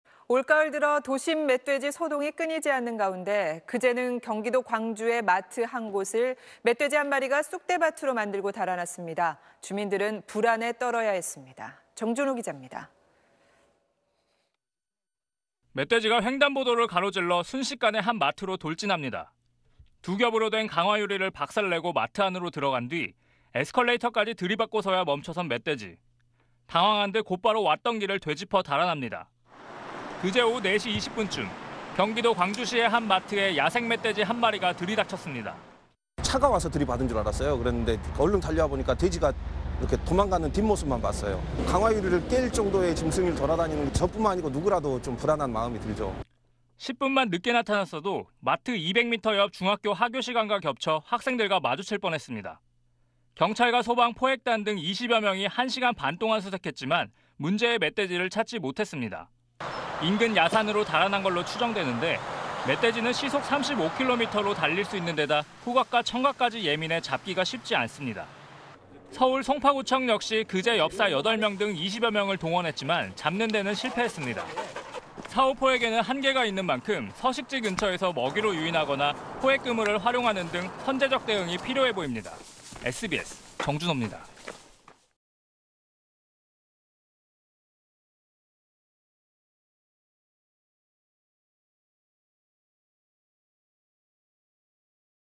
강화유리 뚫고 마트 안 돌진…달아난 멧돼지 어디로? / SBS
올가을 들어 도심 멧돼지 소동이 끊이지 않는 가운데, 그제(22일)는 경기도 광주의 마트 한 곳을 멧돼지 한 마리가 쑥대밭으로 만들고 달아났습니다. 주민들은 불안에 떨어야 했습니다.
정준호 기자입니다.